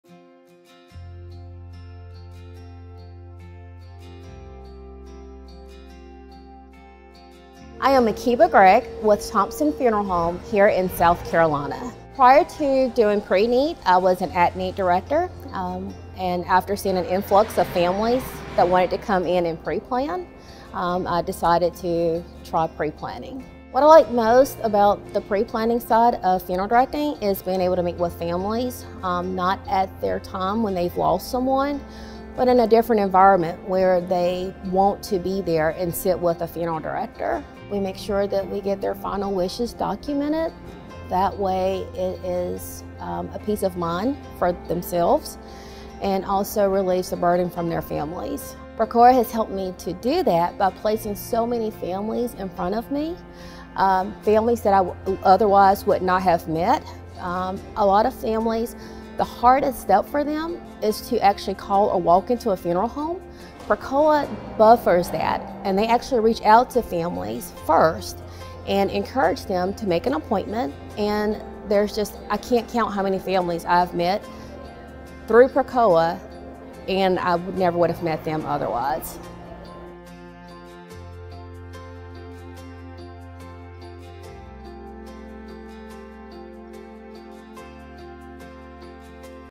Help More Families
Makeba Gregg, Preneed Funeral Director at Thompson Funeral Home, outlines how working in preneed gives you the ability to help more families.